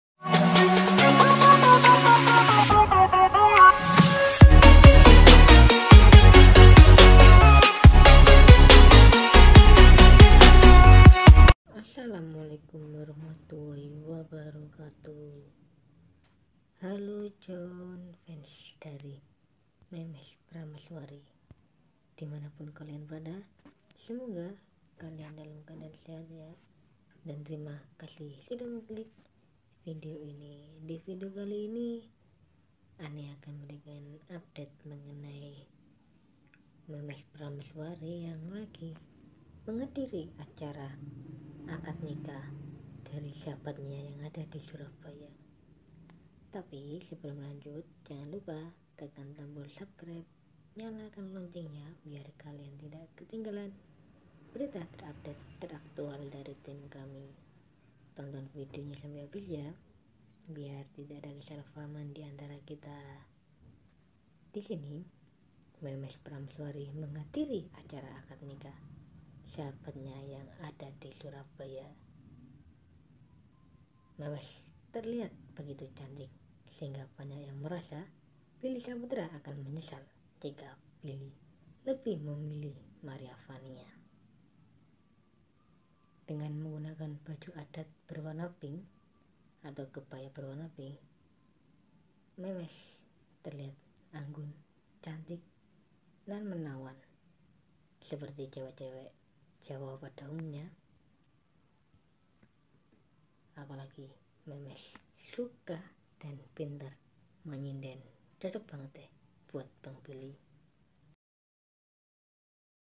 MEMES BAHAGIA SAHABATNYA SUDAH PADA MENIKAH, TINGGAL NUNGGU DI LAMAR SAMA BANG BILLY 😍
memes prameswari bahagia bisa antar sahabatnya menikah dan bahagia sahabtnya pada menikah, tinggal dirinya menunggu dilamar sama bang billy